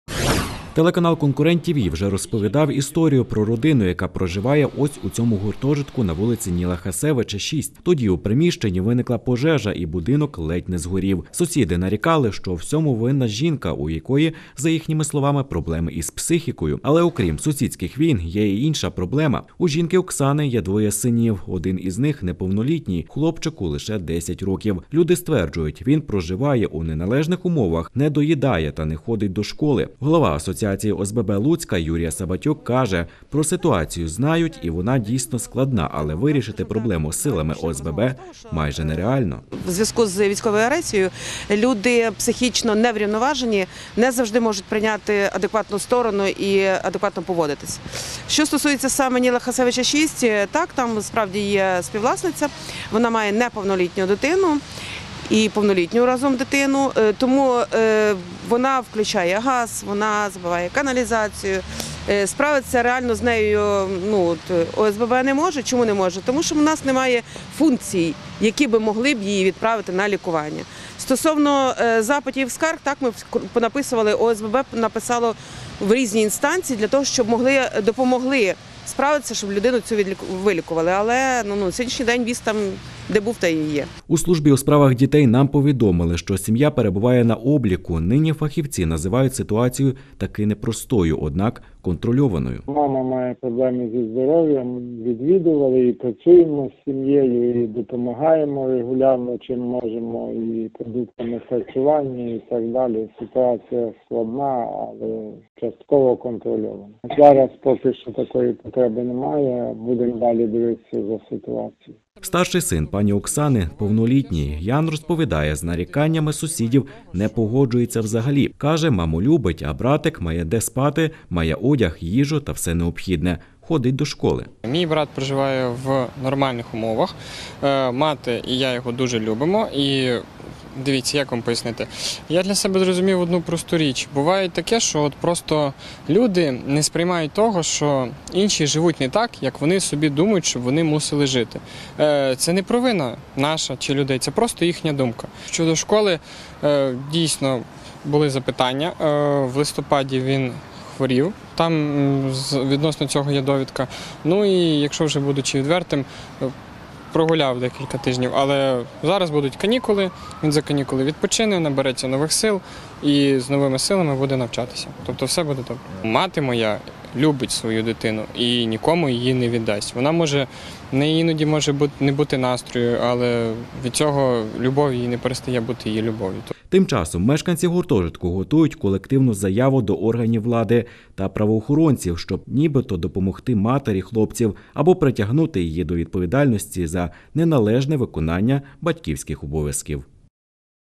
СКАНДАЛ ❗ мешканці луцького гуртожитку готують колективну заяву проти “проблемної” сусідки
Мешканці будинку, що на вулиці Ніла Хасевича готують колективну заяву, аби вирішити питання із проблемною родиною у гуртожитку. Нещодавно у будівлі виникла пожежа. Усі звинувачують у цьому мешканку Оксану, мовляв, у неї проблеми із психікою. Крім того у жінки є малолітній син, який за словами мешканців живе у неналежних умовах. Ми поспілкувалися із родиною Оксани. Що вони кажуть з цього приводу - детальніше у наступному матеріалі.